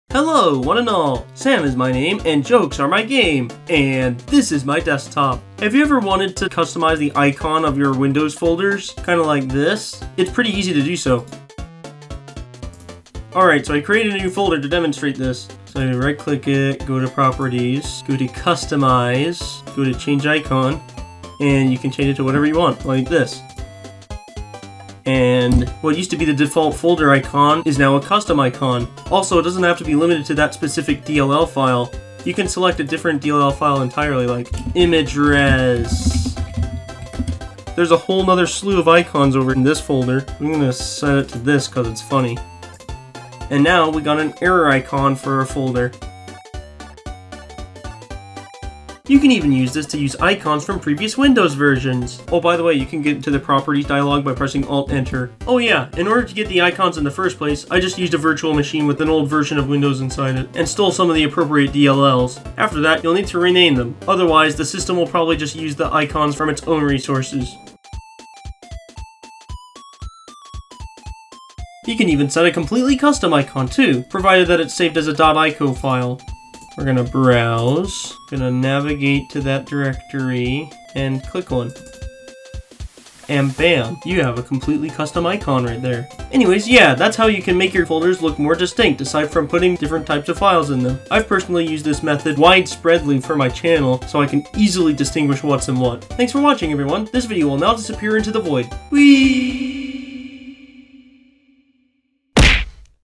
How to customize folder icons in Windows
You can actually make your folders look distinct from each other easily in Windows! Here's several methods how! You can even set your own homemade icons as folder icons (as long as they're saved as .ico files)!
This also works for shortcuts!

└── (channel): 329
Series: Turbo Tutorials
Installment: 5